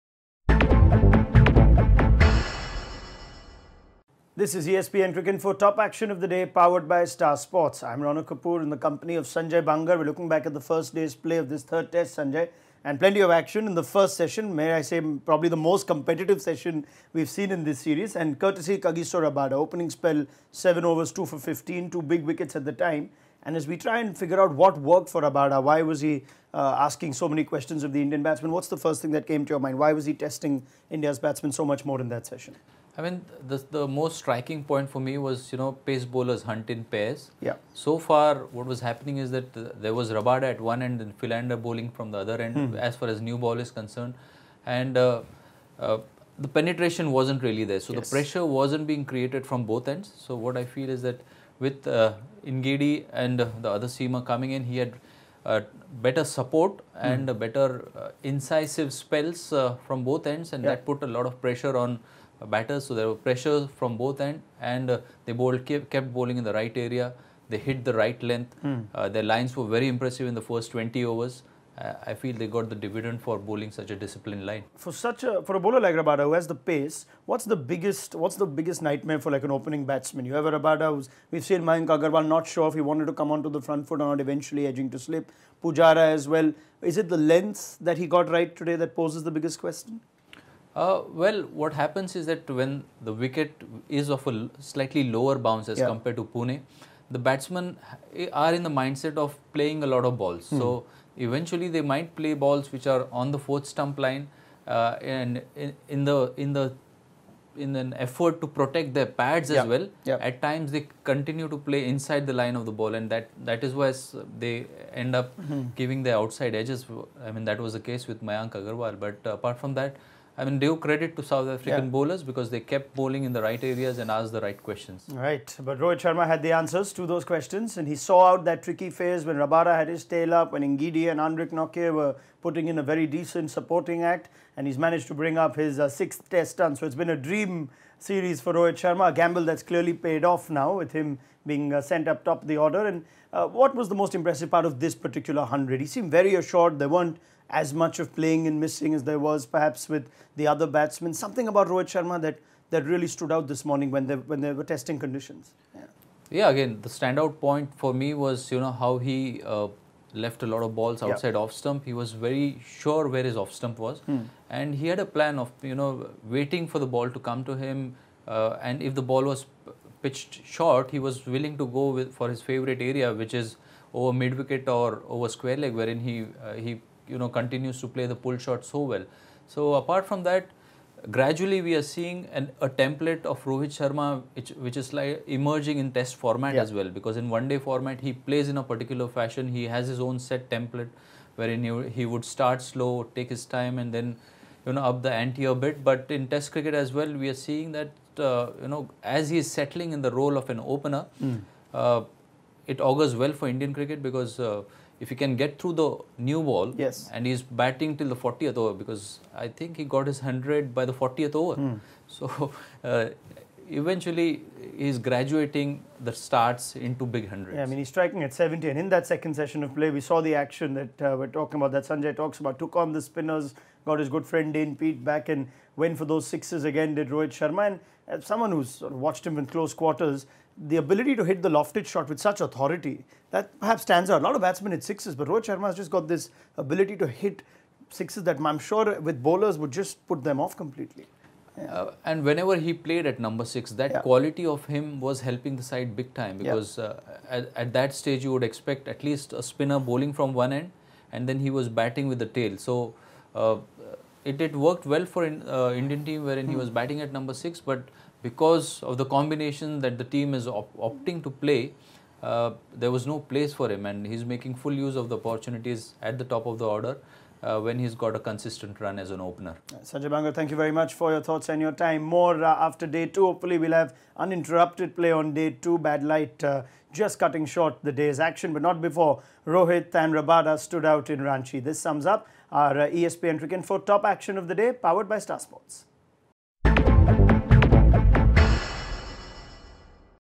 Bangar: Rabada thrived in the support he got with the new ball
Sanjay Bangar reviews the best bit of action from the first day of the Ranchi Test